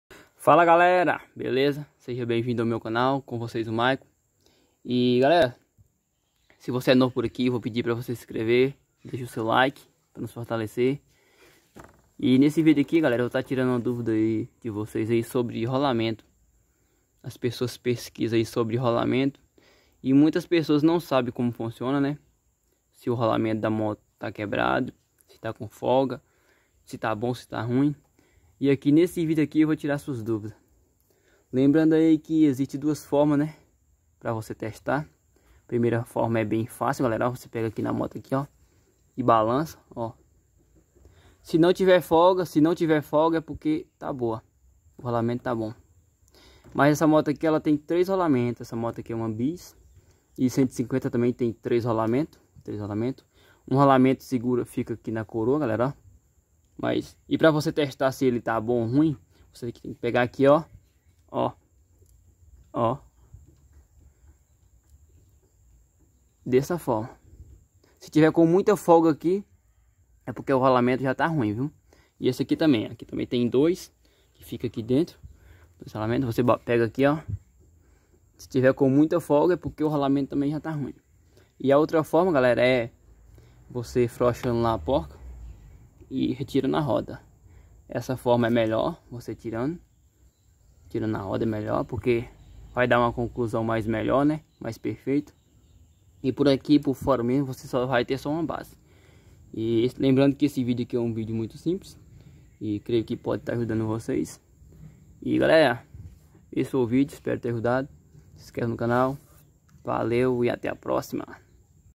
Como Saber se os Rolamentos Estão Ruins
nesse vídeo explicarei tudo a respeito de rolamentos, as pessoas tem dificuldade, e não sabe se os rolamentos estão bons ou ruins, nesse vídeo tirarei as suas dúvidas.

Maycon Henrique

tudo sobre motos

QUER APRENDER A GANHAR DINHEIRO USANDO A INTERNET?
Acessa esse link agora e mude sua vida.